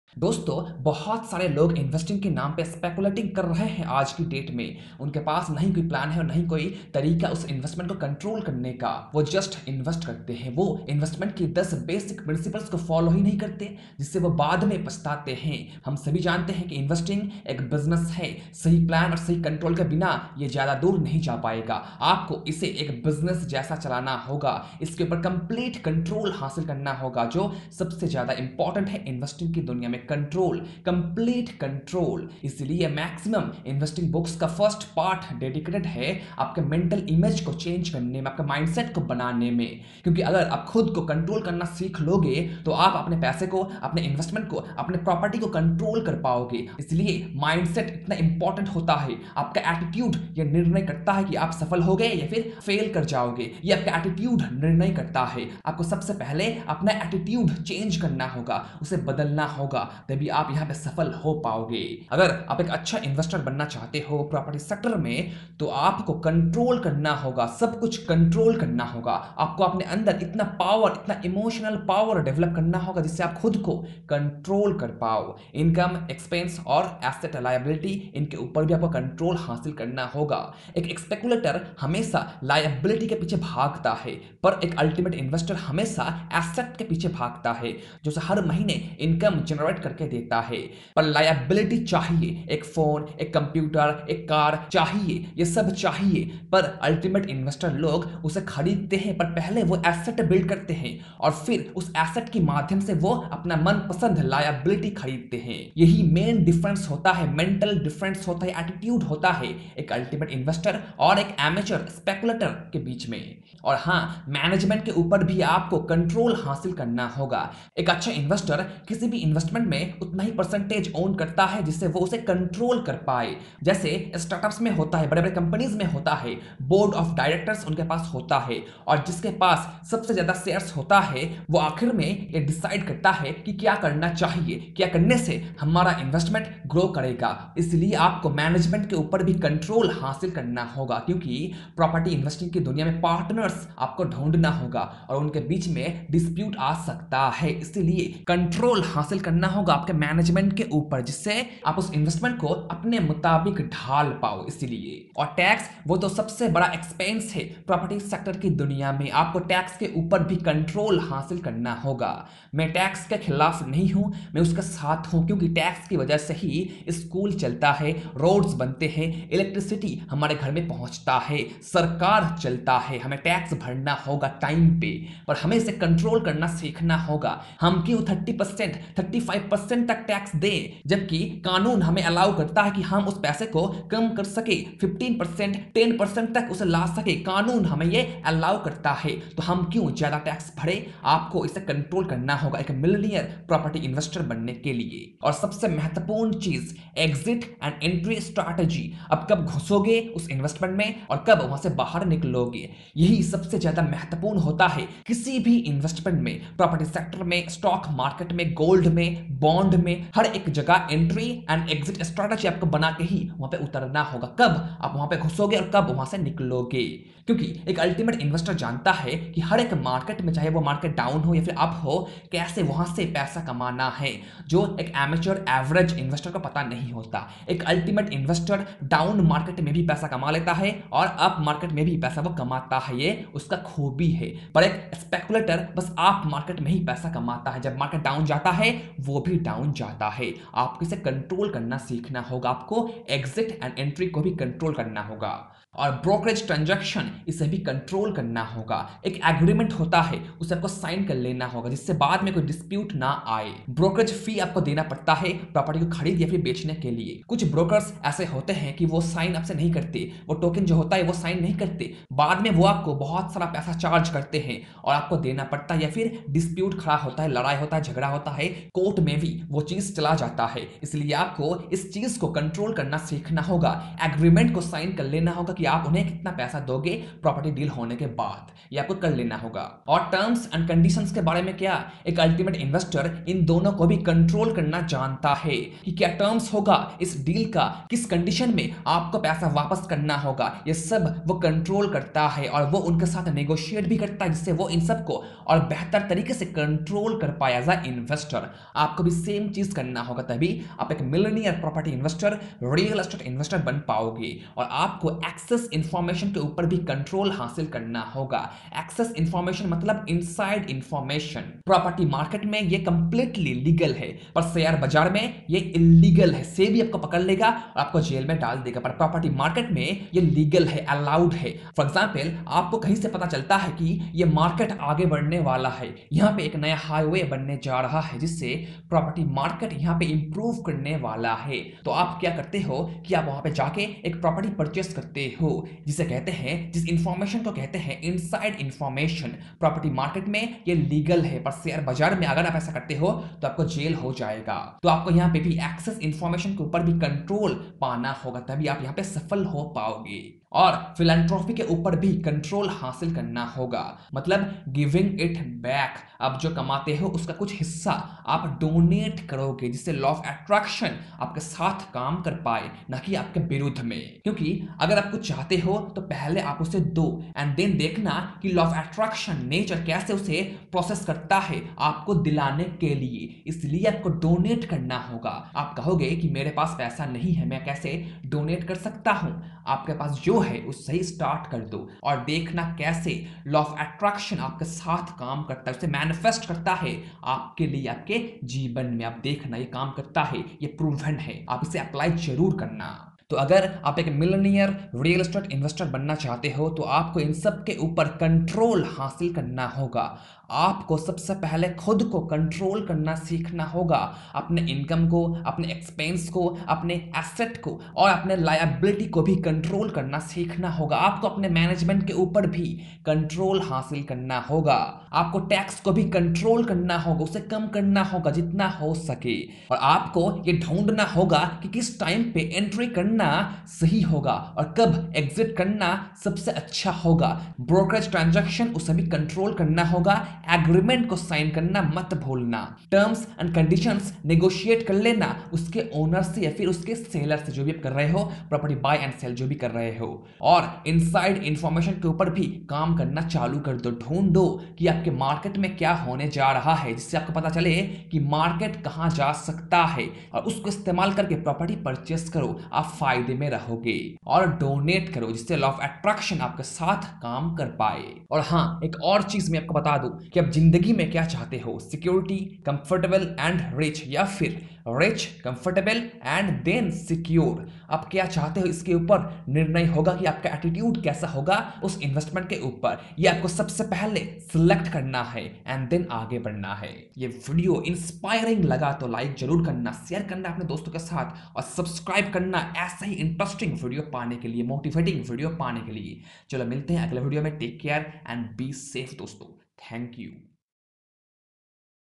10 steps to become a millionaire real estate investor
Here are the ten steps you need to take to become a millionaire real estate investor. I am not gonna say it will be easy, but if you stick to it shall work. Most successful real estate investors know the real difference between investing and speculating and you should know it too.

Because investing is not what you think. The game is very different from what everyone is talking about, especially in real estate. This video will open your mind and your eyes.

Books I love:

📚  Millionaire Real Estate Investor
📚  ABC of Real Estate
📚  Real Book of Real Estate
📚  Wealth of the Nation

Sheikh Najmus

Jai Hind 🇮🇳